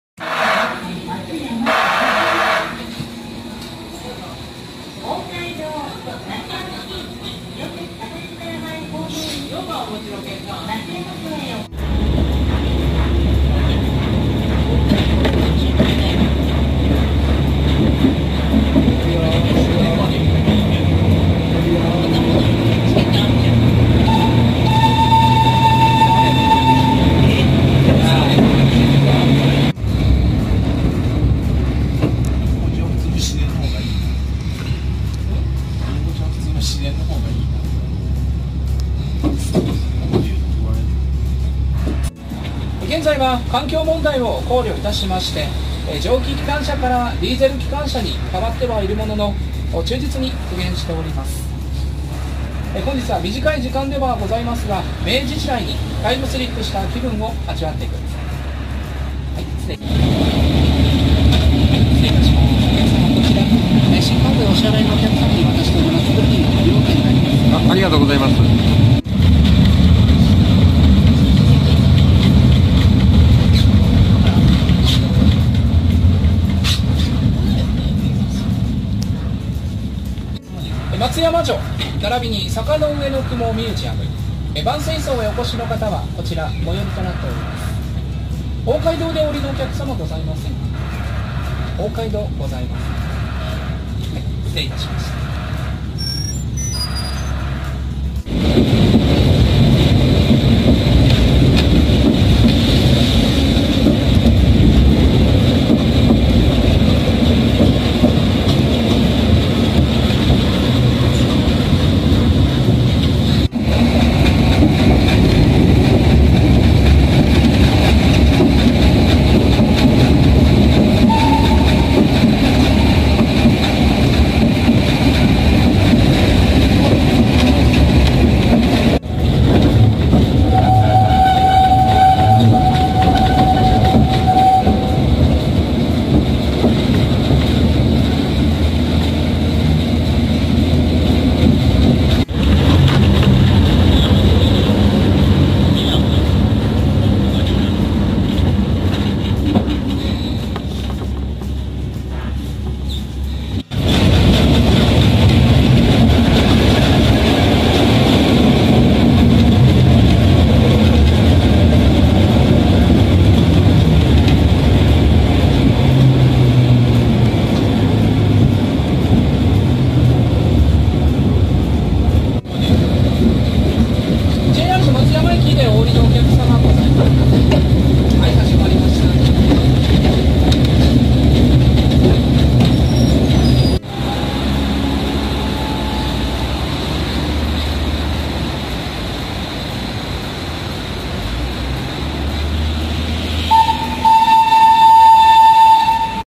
坊っちゃん列車で道後温泉駅から松山駅前駅まで行きました！
今回は、坊っちゃん列車で道後温泉駅から松山駅前駅まで行きます！道後温泉→大街道→松山駅前の順に停車します。JR松山駅前を発車する坊っちゃん列車